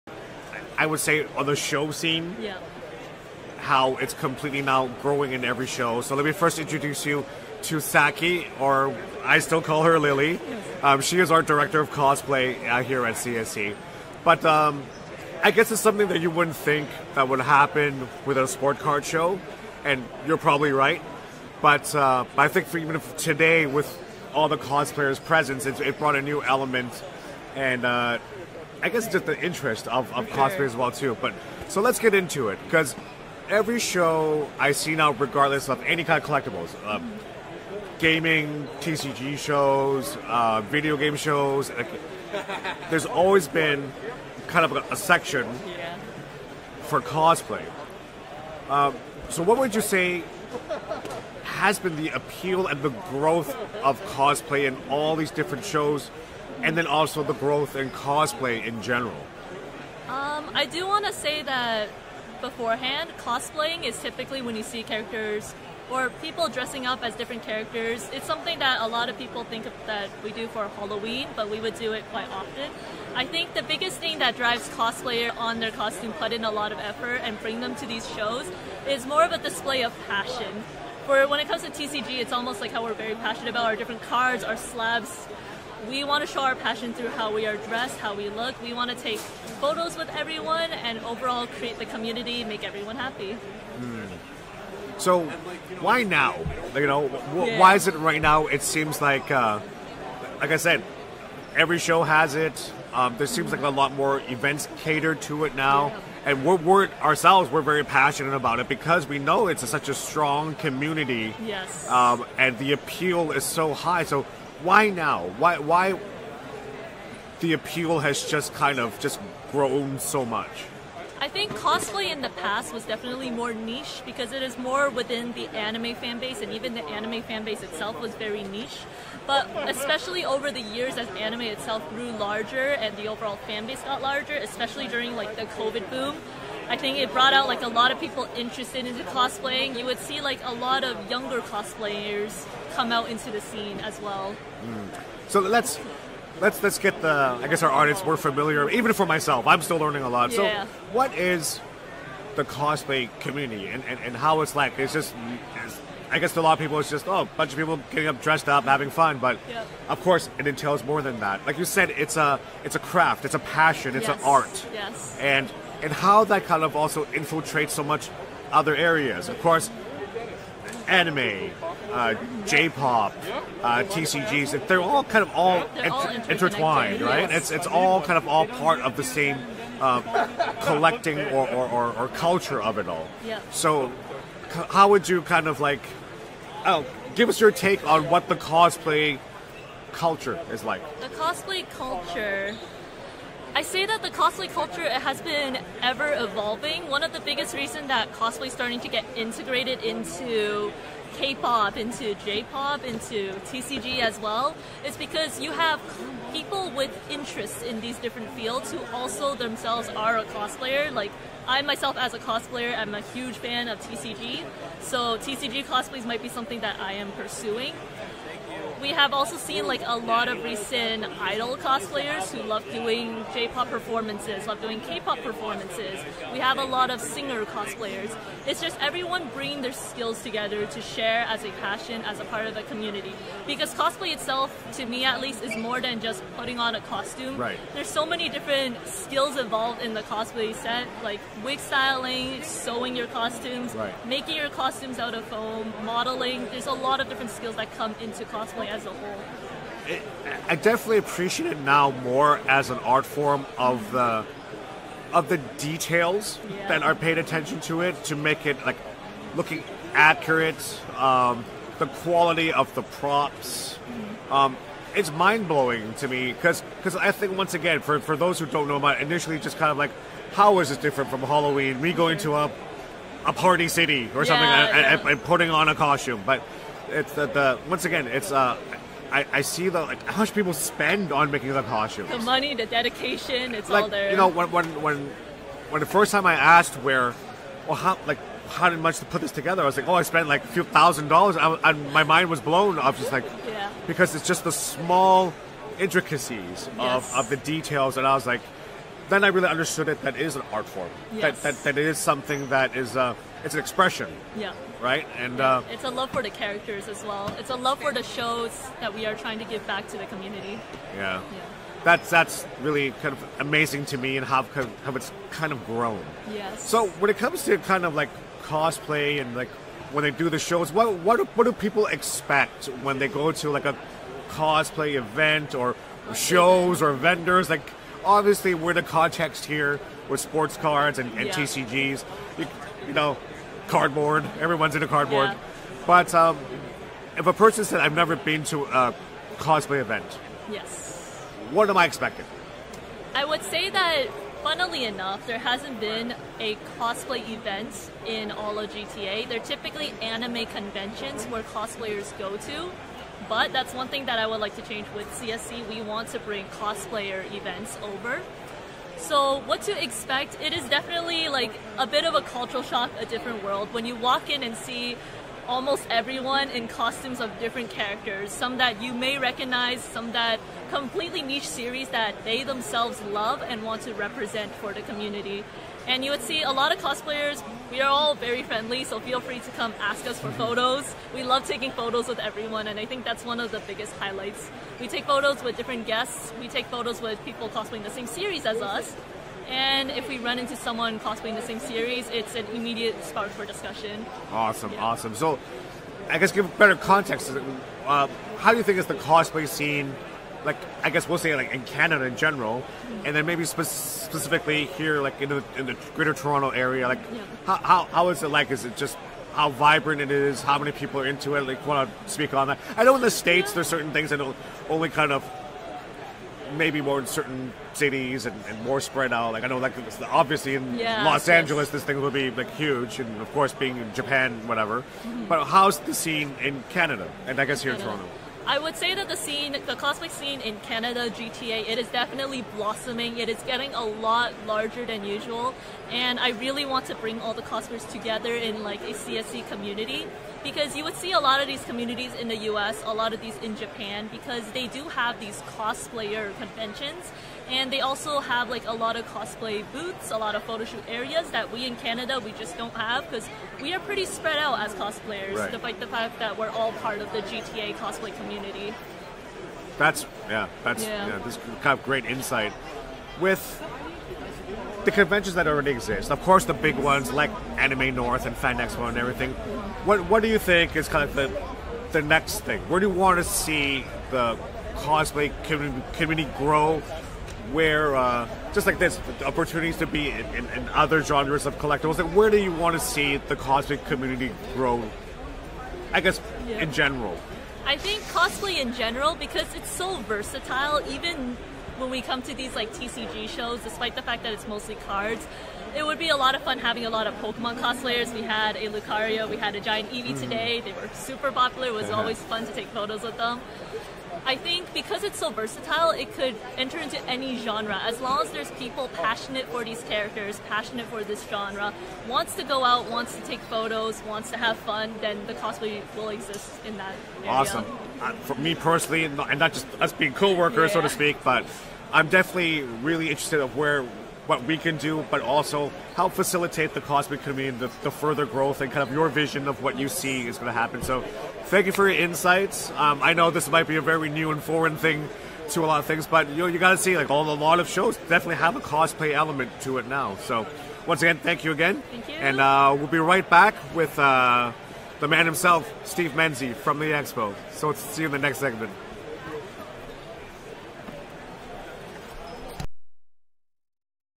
2023 Toronto Sport Card Expo Interview about Cosplay (CSC + Sakii)
CSC interviews Sakii about the rise of Cosplay